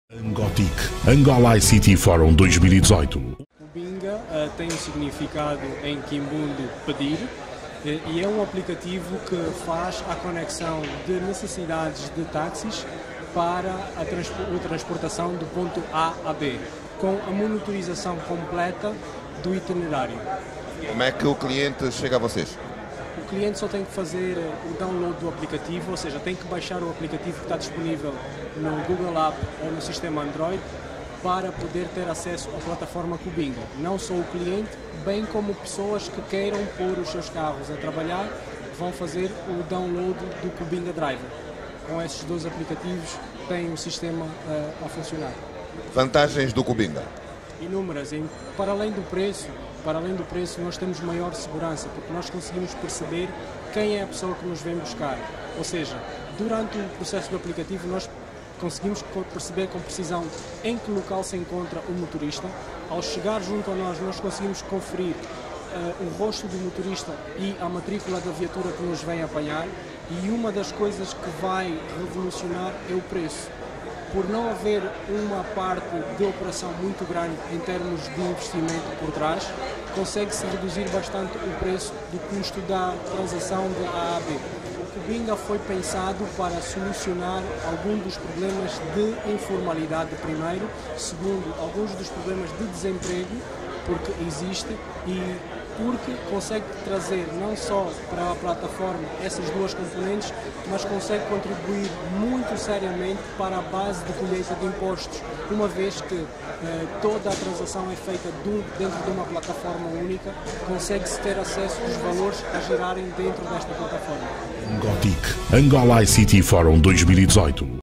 ANGOTIC - Angola ICT Forum 2018. Entrevista Com Emerson Pain.
O ANGOTIC - Angola ICT Forum 2018 é um evento de tecnologias de informação e comunicação de referência na região de África, realizado este ano em Luanda - Angola, de 19 à 21 de Junho de 2018. Este fórum é a plataforma ideal para se actualizar das últimas tendências e standards de indústria, em um ambiente de networking internacional entre profissionais das TICs, empresas, entidades governamentais, investidores e empreendedores.